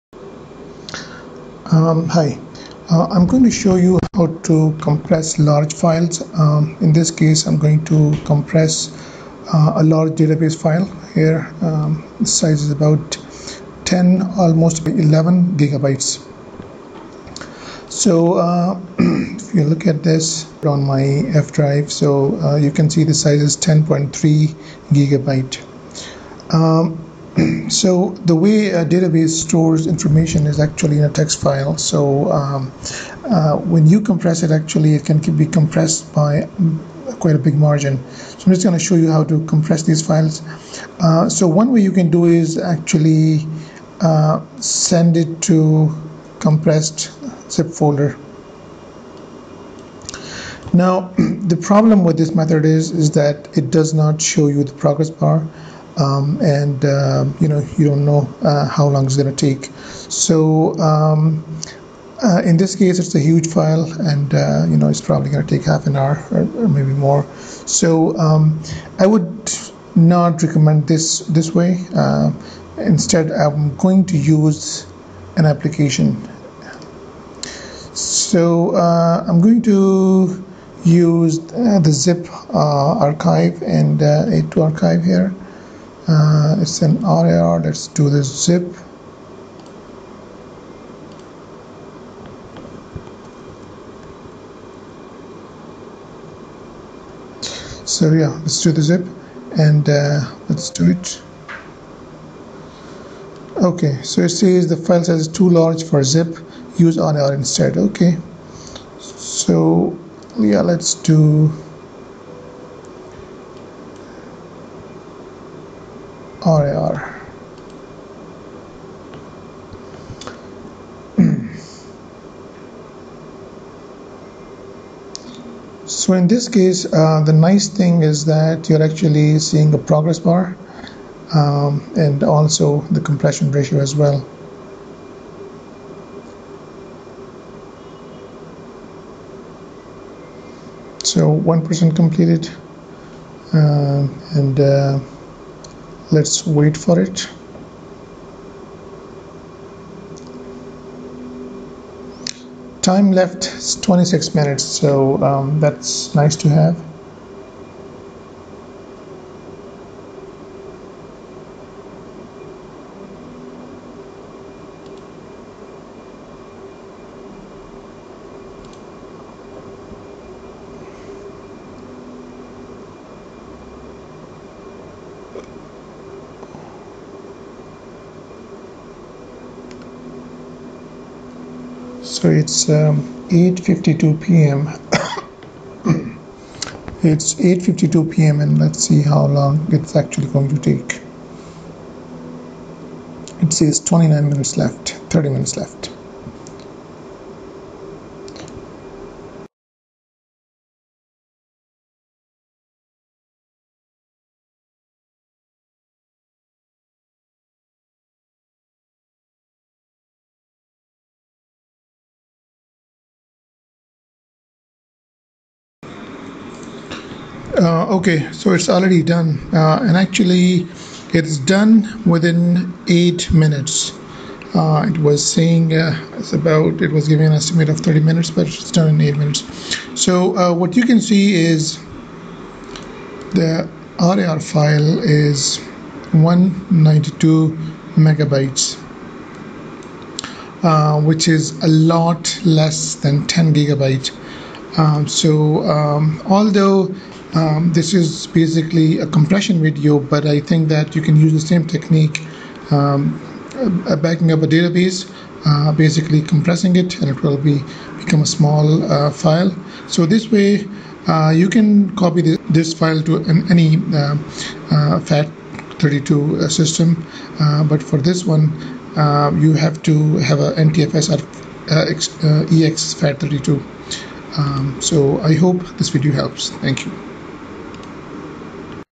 How to backup large database files using compression
You do not need to worry about trying to find how to backup large files. Simply compress them ( .db and .log files). These files will be compressed by 98% and will take very less space on hard disk. In my case 10GB file was compressed to 200MB,  exact compression

Uncompressed File: 10,904,768 KB
Compressed File:  201,602 KB

which gives compression ration of 98%